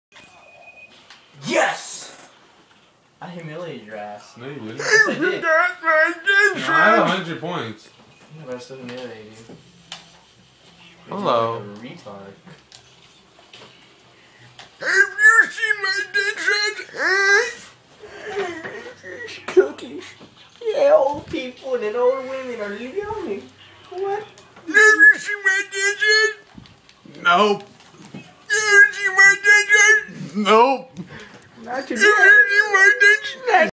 Have you seen my Dentures?
An old senile man searches for his dentures.